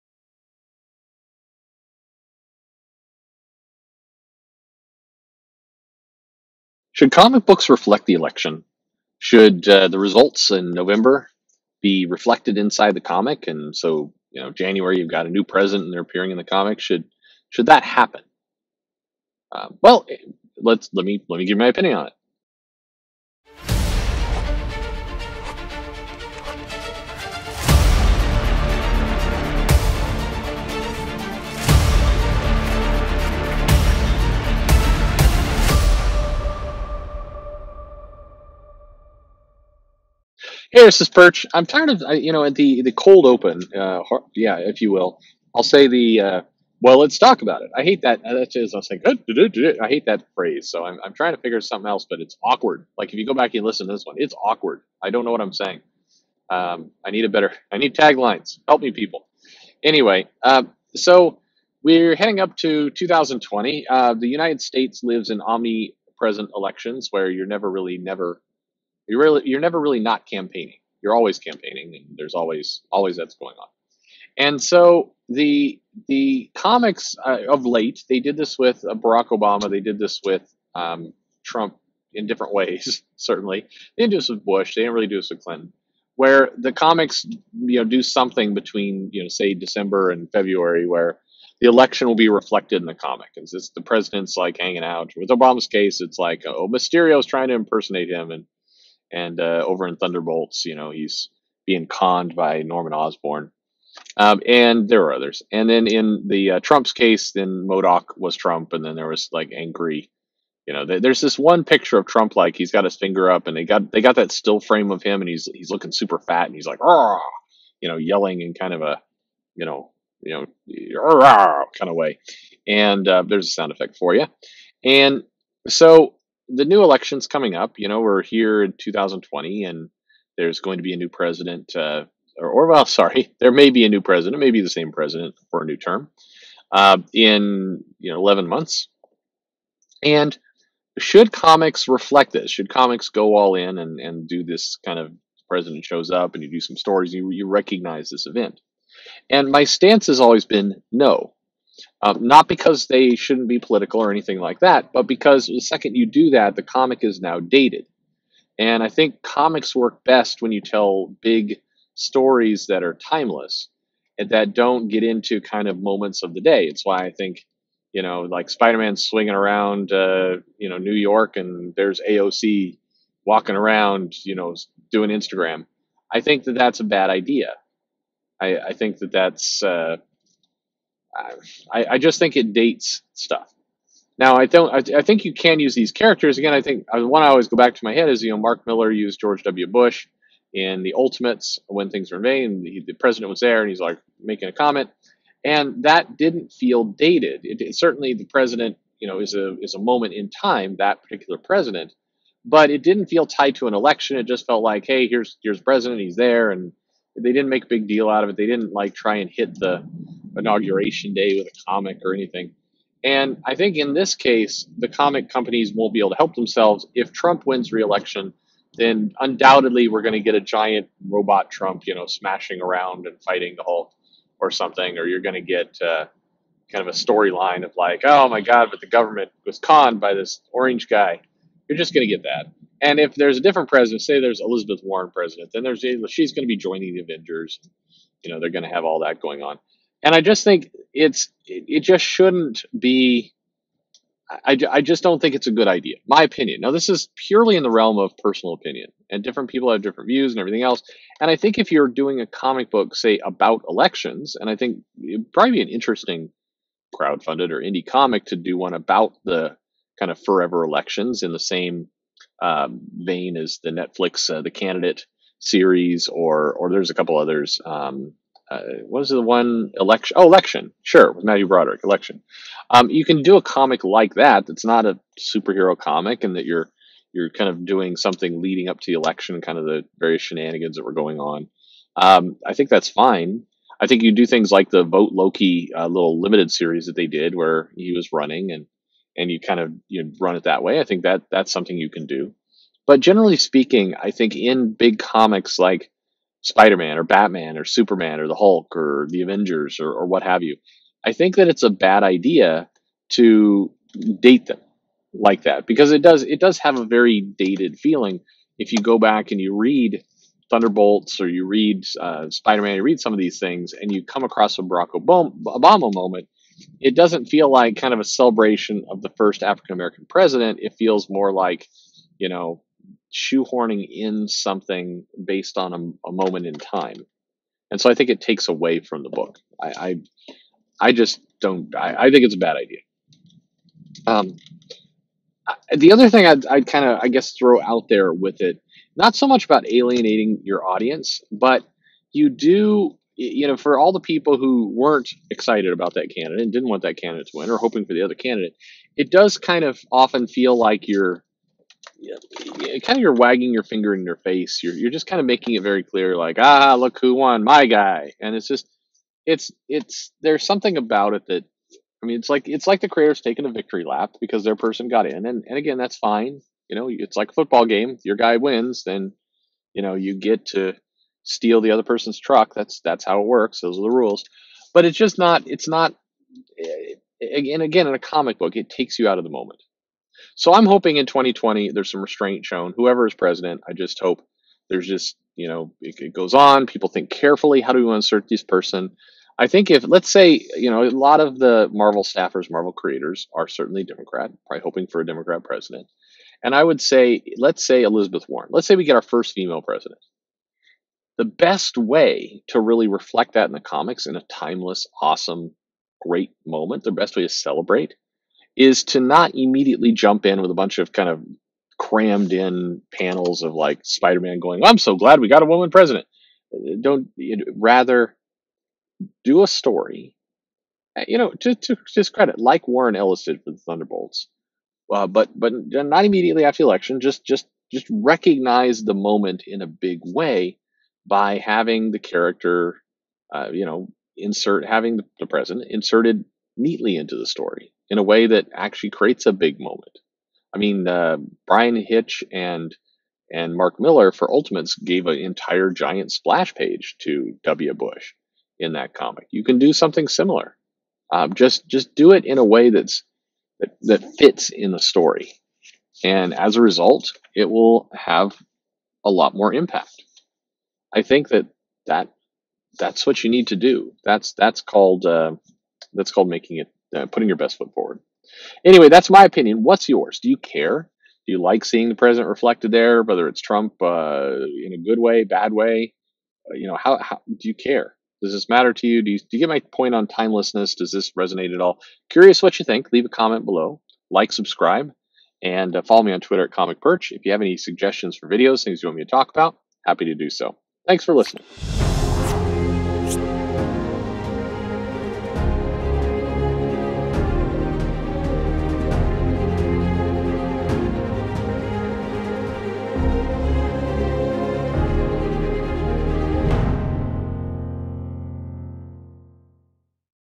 Should President Trump Be Featured In Comic Books?
Creator's note: this video is discussing the use of President Trump (and other political figures) in mainstream comics like Batman, Spider-Man and others... and not parody comics or comics whose purpose is to specifically handle those topics. There's a major difference between a "My Hero MAGAdemia" and Trump showing up as MODOK in a Marvel comic. For what it's worth, the work by Tim Lim, Mark Pellegrini, Brett Smith and many others who produce witty, on-point political books and commentary are always welcome and well done.

With so much attention riveted to politics, it's tempting to include current figures like President Trump within your comics. But is it a good idea? Does it create extra buzz for the book, or forever date the comic to a particular moment in time? Regardless of if the representation is good or bad, why using current political figures may not be the best move.